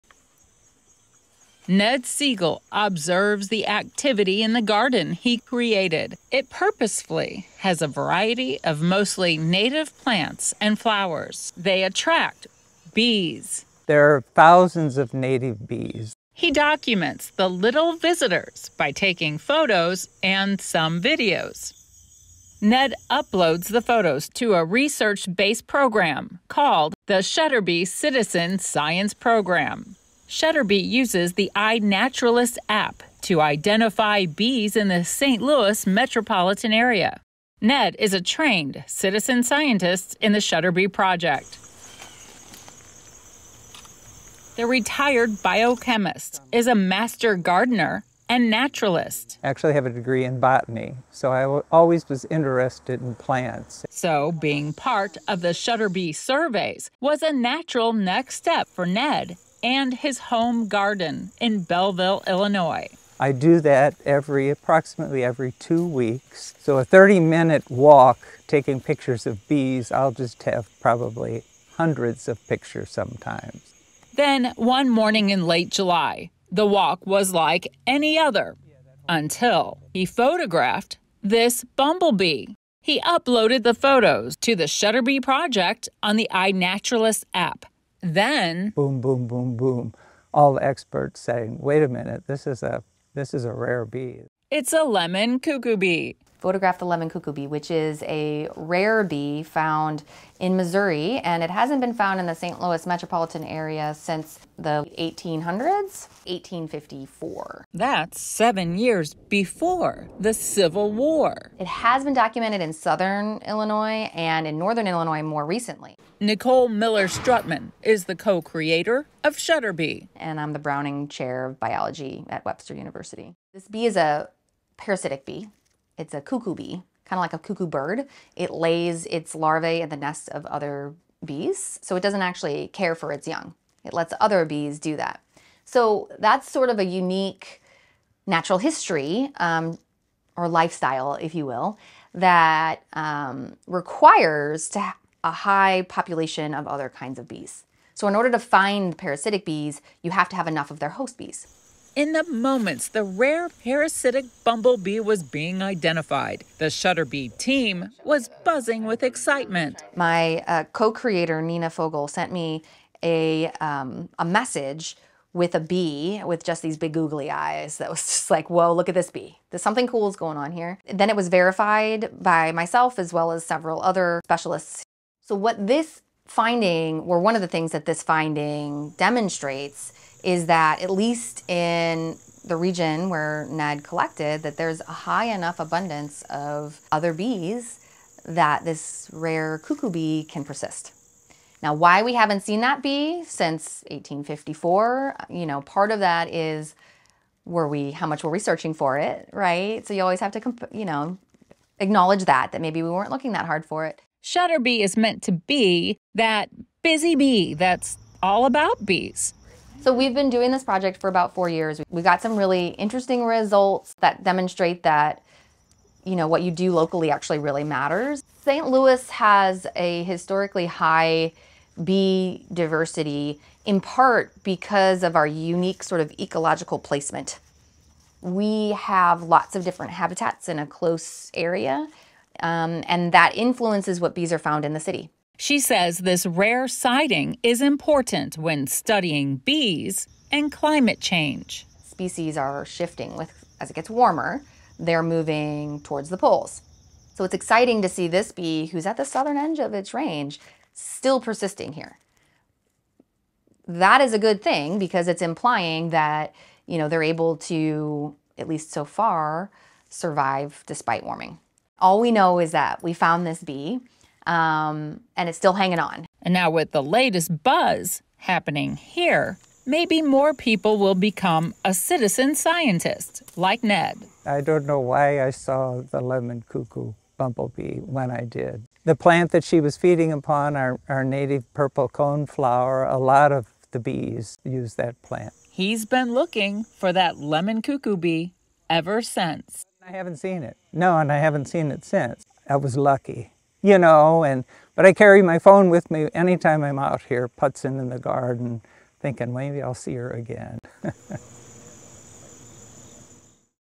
'Citizen scientist’ finds rare parasitic bumble bee identified through the Shutterbee Project
Ned Siegel of Belleville, Illinois observes the bee activity in the garden he created, that purposefully has a variety of mostly native plants and flowers. They attract a variety of bees. He documents the arrival of the little visitors by taking their pictures!
 Siegel uploads the photos to a research-based program called the Shutterbee Citizen Science Program. Shutterbee uses the iNaturalist app to identify bees in the St. Louis metropolitan area.
Then one morning in late July, Siegel photographed one bumble bee creating a loud buzz among the experts.
“Boom, boom, boom, boom! All experts saying, ‘Wait a minute. This is a rare bee!”
 Webster University Biology Professor Nicole Miller-Struttmann, co-creator of Shutterbee, said Siegel photographed a rare parasitic bumble bee. It’s the Lemon Cuckoo bumble bee.
“Which is a rare bee found in Missouri,” said Miller-Struttman, co-creator of Shutterbee. “And it hasn't been found in the St. Louis metropolitan area since the 1800s, 1854.”